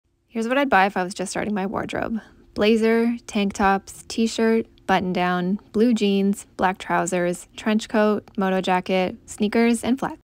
10 closet staples I’d buy if I was just starting my wardrobe ✨
These are the top 10 clothing items I'd buy if I was just starting from scratch. Feel free to add colour, texture, patterns and your own personality as you see fit!

Items mentioned:
tshirts:
*organic basics boxy cotton tee (s) - STYLEXB to save 10%

tank tops:
*organic basics cotton rib flex tank